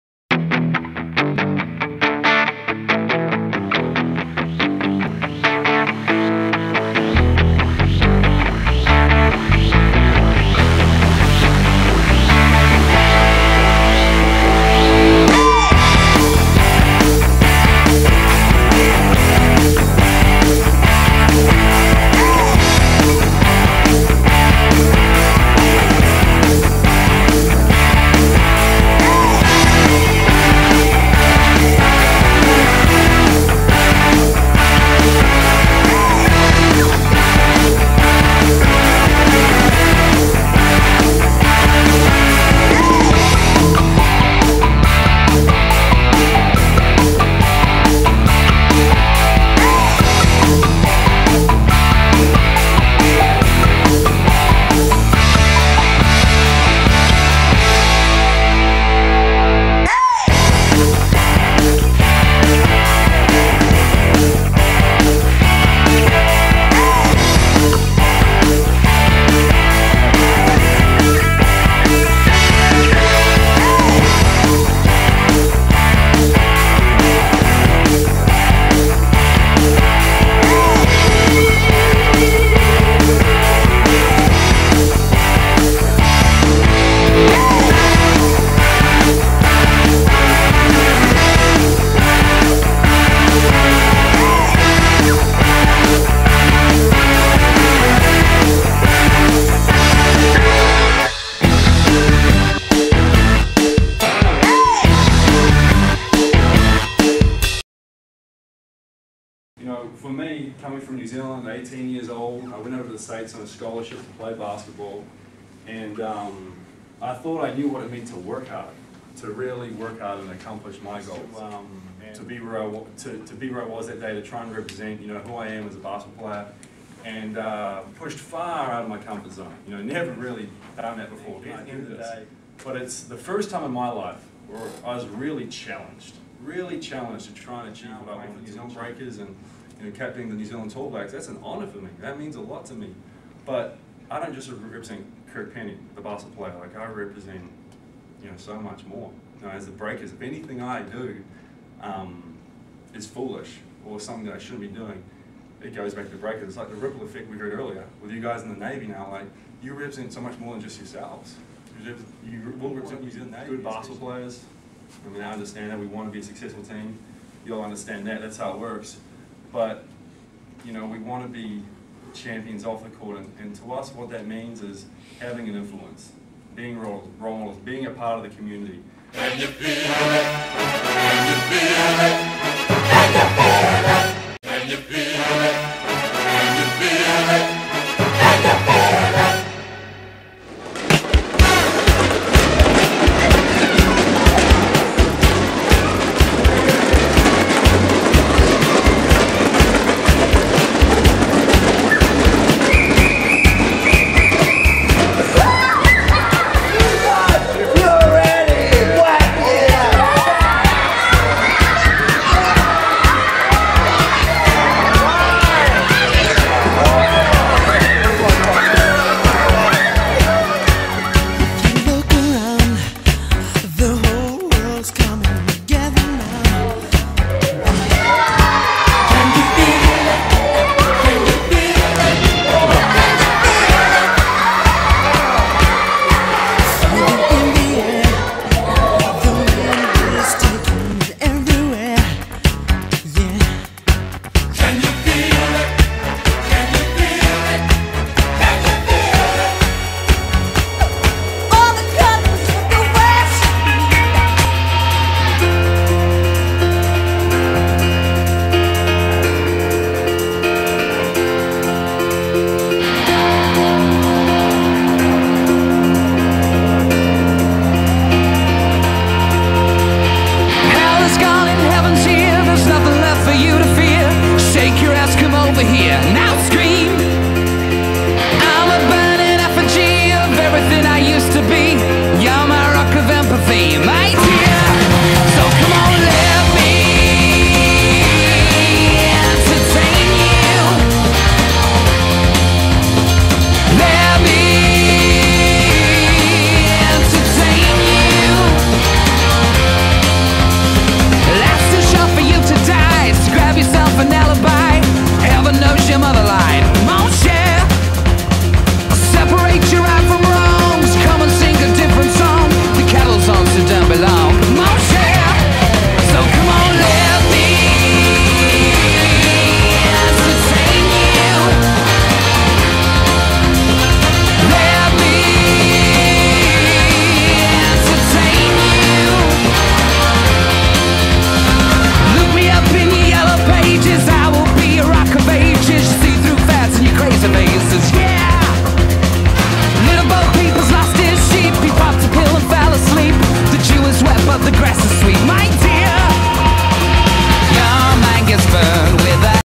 Breakers Week #2
The Burger King Breakers defeated the 36ers in overtime, 99-91, what a thrilling game! Kirk Penney was invited to the Navy base to speak in their mentoring workshop, while the rest of the team kept on doing community work -playing a Breaker Ball in Henderson.